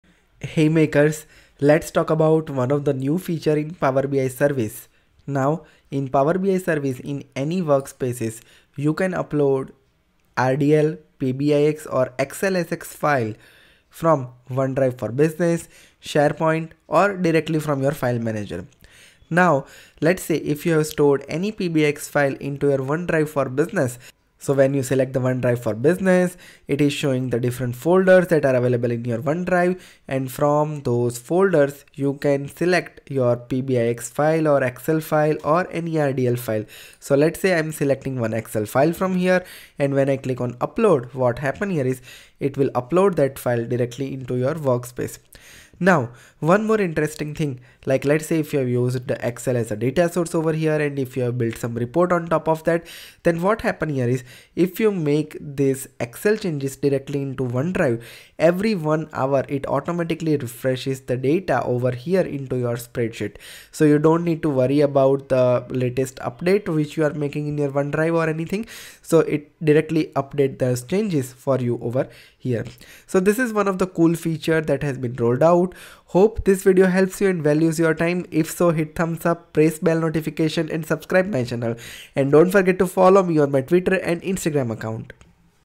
New way to Upload File from Power BI Workspace
In this short video, let's talk about new way to upload files from Power BI Workspace. In the workspace you want to add files to, you’ll see an Upload option next to the New button. You’ll be able to use this dropdown to connect to files stored in OneDrive for Business or any SharePoint site you have access to or upload them from your computer through the Browse option.

Chapters:
00:00 Start
00:40 Upload File from OneDrive for Business
01:00 Data refresh from OneDrive for Business